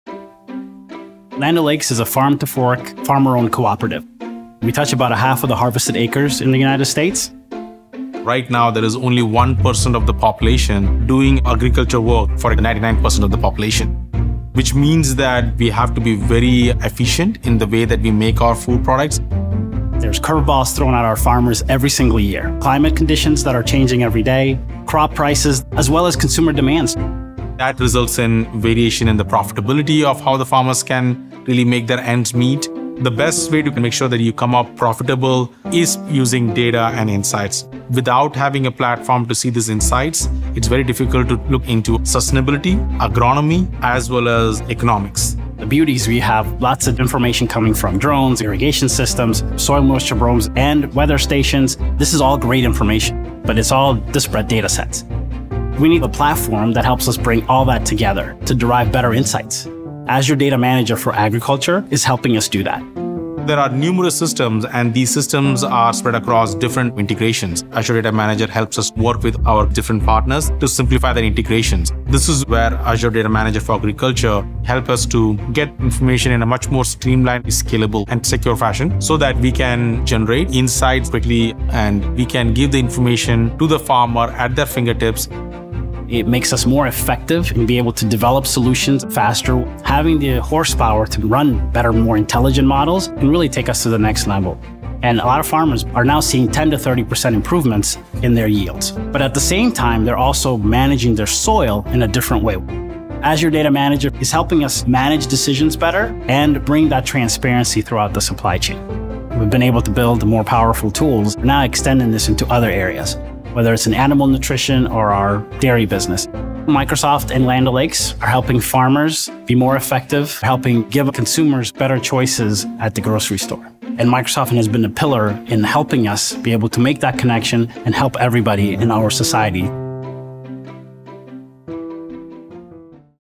Land O’Lakes uses Azure Data Manager for Agriculture to support efficient farming
Land O’Lakes is one of the largest farmer-owned cooperatives and touches half the harvested acres in the United States. Farmers are integral to all its businesses including WinField United crop inputs and insights, Animal Nutrition, Dairy Foods and Truterra. Land O’Lakes turned to Azure Data Manager for Agriculture to provide growers with data-driven insights that make them more efficient, profitable, and sustainable in their practices.